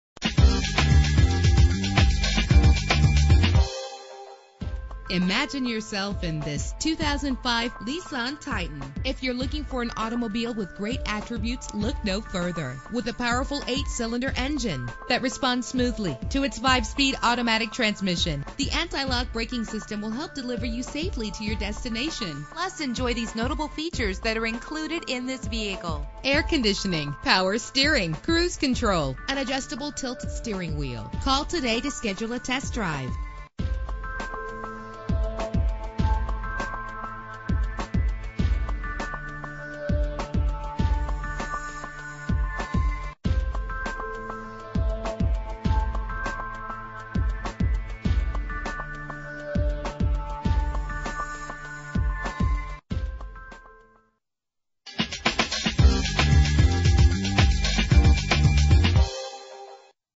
2005 Nissan Titan - Dallas TX
Year: 2005
Make: Nissan
Model: Titan
Trim: T
Engine: 5.6 liter 8 cylinder 32 valve
Transmission: 5-Speed Automatic
Color: Gray
Mileage: 62258
Address:
11550 Lyndon B Johnson (635 E)